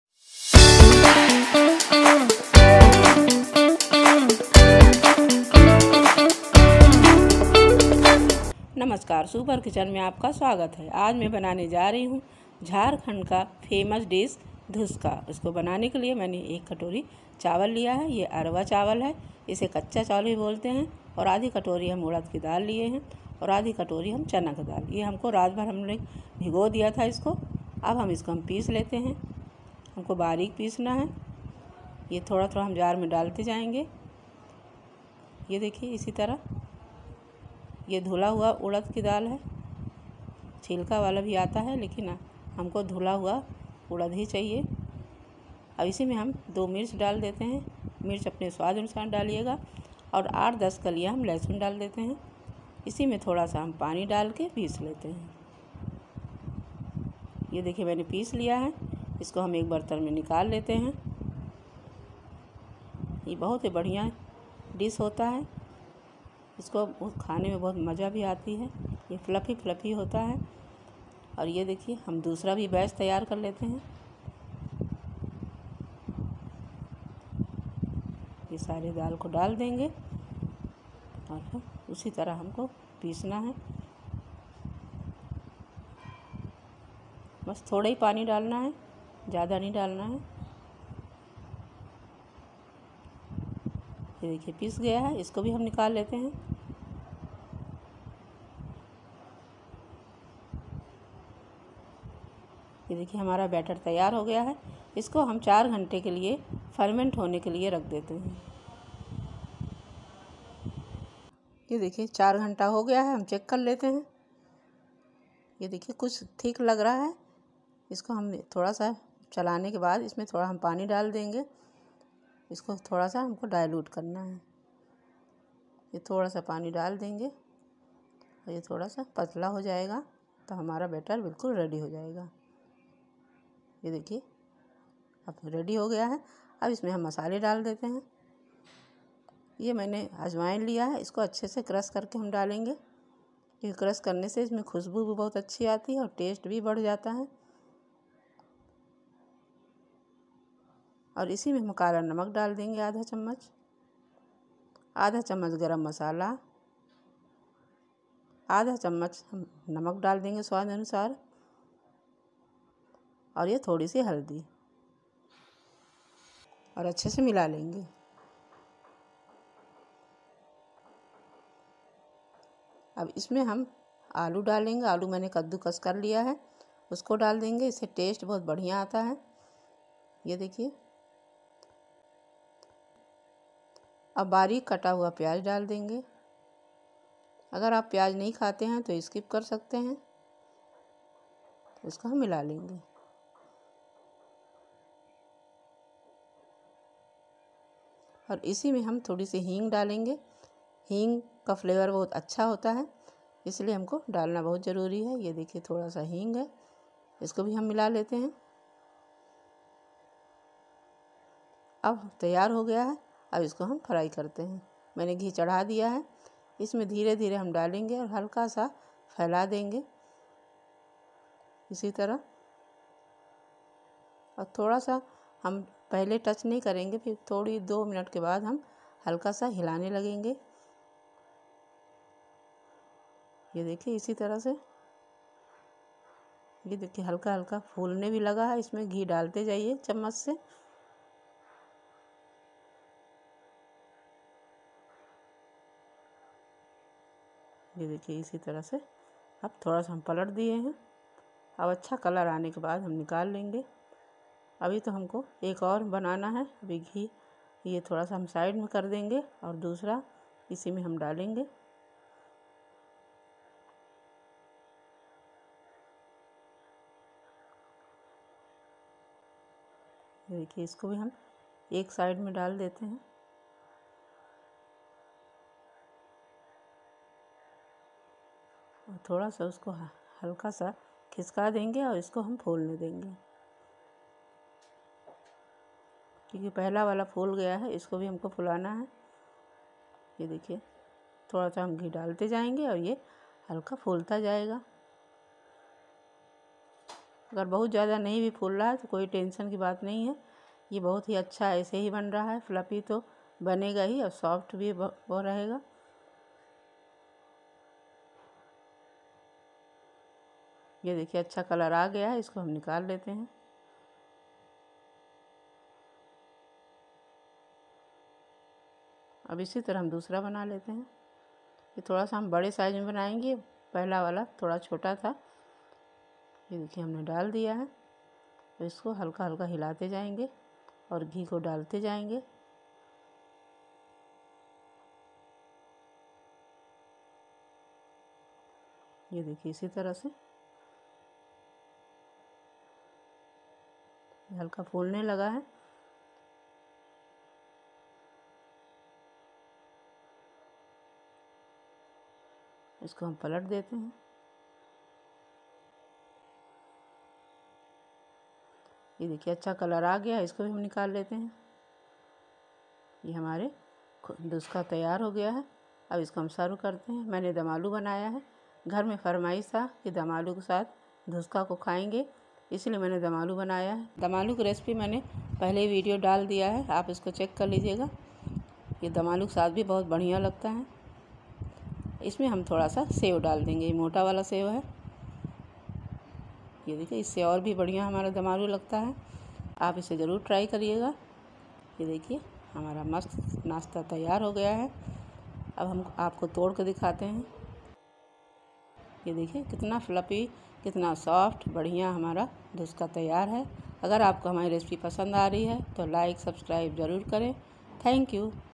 Dhuska Special Recipe 😋 || Super Kitchen
Aaj is video me, main aap sabhi ke sath dhuska ki recipe share karne ja rahi hu, kripya is video ko end tak jarur dekhe.